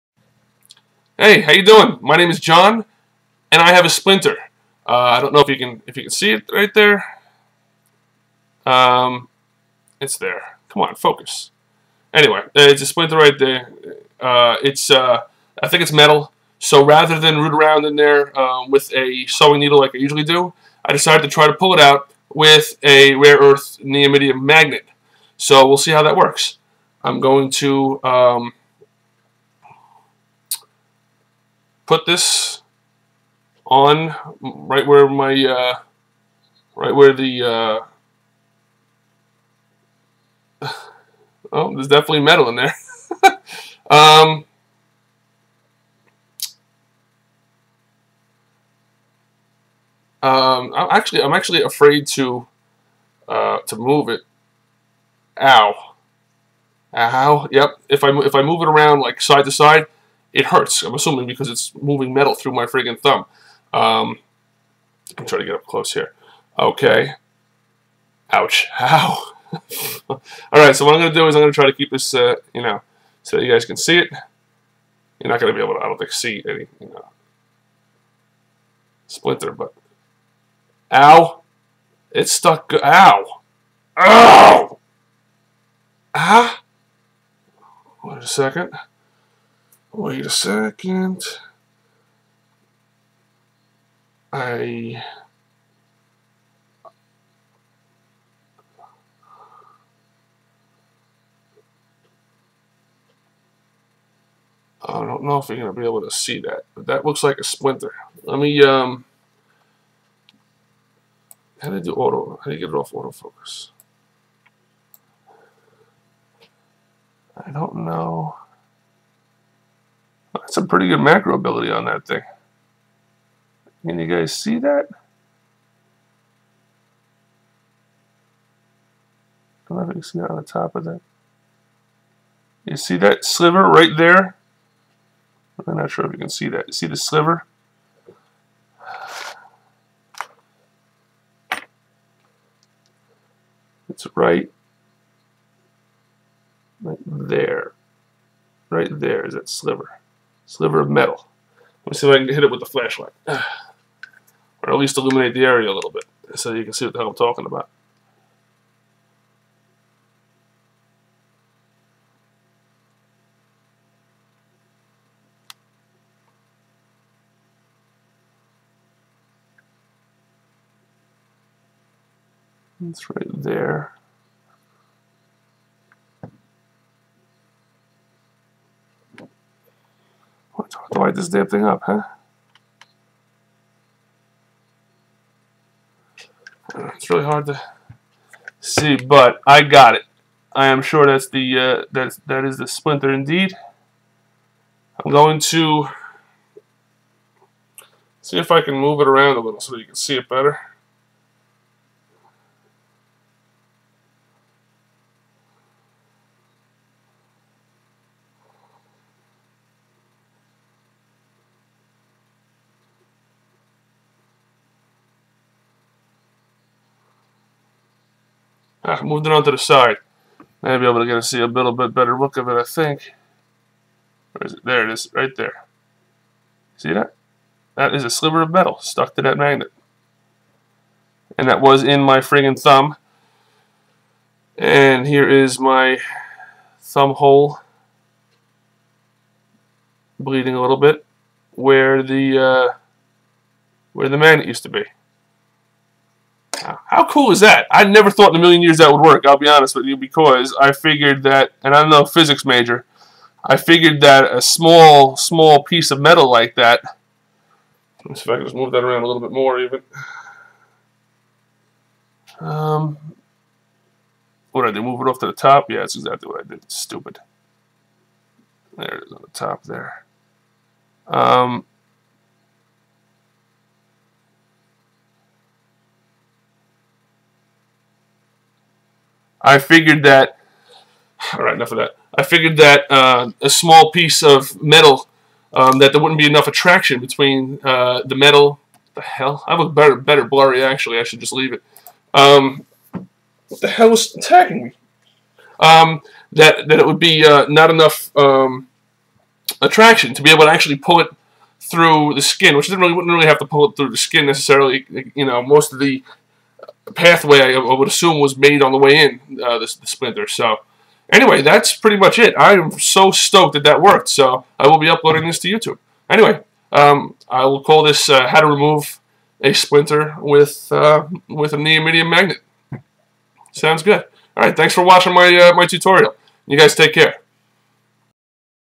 How to remove a metal splinter with a neodymium magnet
Sorry for butchering the pronunciation of the word neodymium.  I don't usually pronounce that wrong and didn't notice until I put the video up.  I feel like a dick.  Anyway, I hope you enjoy the video, if you have any questions please post them in the comment section.

Thanks.

How to remove a metal splinter with a neodymium magnet.  In this video I show you how I removed a sliver of metal from my finger using a neodymium magnet.  Like I said in the video, I am almost shocked it worked.  The splinter was not poking through, and skin had grown over part of the entry wound.  I initially had picked at it with a sterilized needle for a little while with no luck, and I tried to work it out by applying careful pressure with my other fingers, but that didn't work either.  So I let it sit a couple days, hoping it would work it's way to the surface by itself, and it did not.  Then I tried this and voila, it worked.  I did not think there would be enough attraction to be able to pull it through skin, but I imagine that some of the initial entry pathway was still there and that that helped.